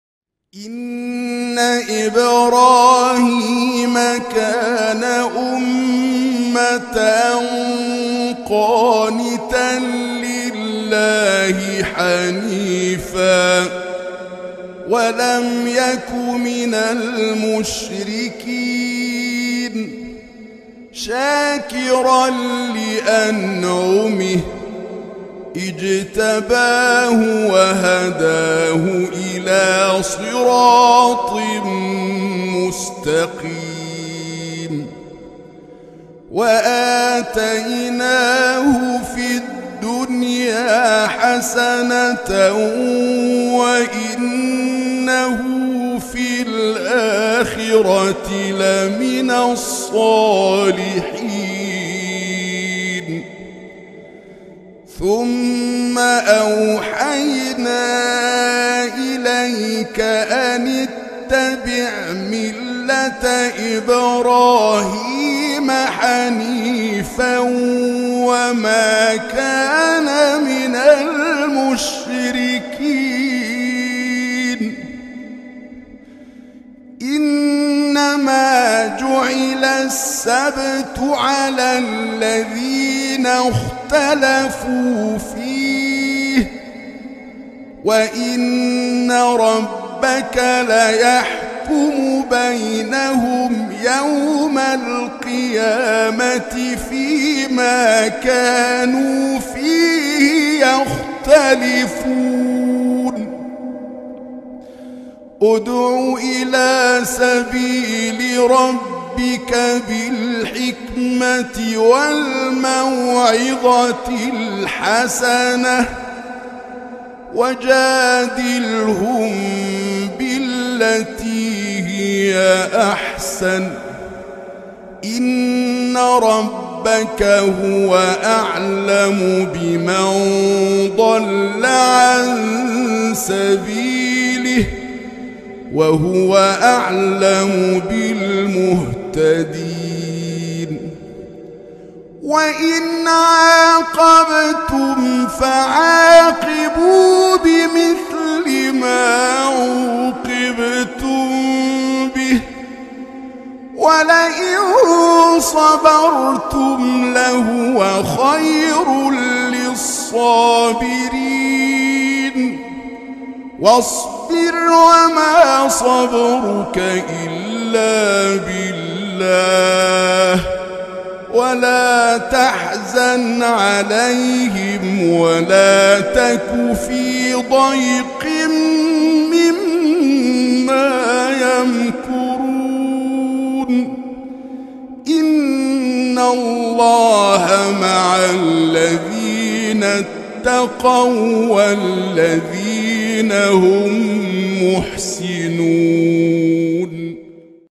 Ан-Нахль | 120-128 |🎙 Чтец - الشيخ محمود الطوخي
📖 Ан-Нахль  | 120-128 |
🎙 Чтец - الشيخ محمود الطوخي

120
Воистину, Ибрахим (Авраам) был вождем, покорным Аллаху. Он был ханифом и не был одним из многобожников.
121
Он был благодарен Аллаху за благодеяния, и Он избрал его и повел прямым путем.
122
Мы даровали ему добро в мирской жизни, а в Последней жизни он будет в числе праведников.
123
Затем Мы внушили тебе: "Исповедуй религию Ибрахима (Авраама), ведь он был ханифом и не был из числа многобожников".
124
Воистину, суббота была предписана только тем, кто впал в разногласия по этому поводу. Воистину, твой Господь в День воскресения рассудит между ними в том, в чем они расходились во мнениях.
125
Призывай на путь Господа мудростью и добрым назиданием и веди спор с ними наилучшим образом. Воистину, твой Господь лучше знает тех, кто сошел с Его пути, и лучше знает тех, кто следует прямым путем.
126
Если вы подвергните их наказанию, то наказывайте их так, как они наказывали вас. Но если вы проявите терпение, то так будет лучше для терпеливых.
127
Посему терпи, ибо твое терпение — только от Аллаха. Не скорби по ним и не печалься оттого, что они ухищряются.
128
Воистину, Аллах — с теми, кто богобоязнен и кто творит добро.